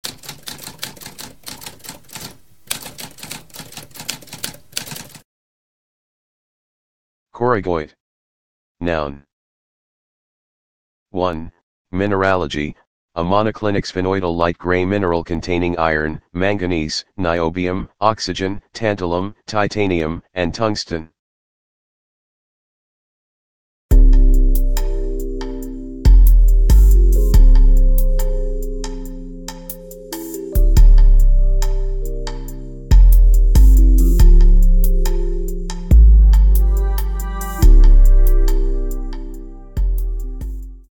What does koragoite mean?
A spoken definition of koragoite.

Intro Sound:
Typewriter - Tamskp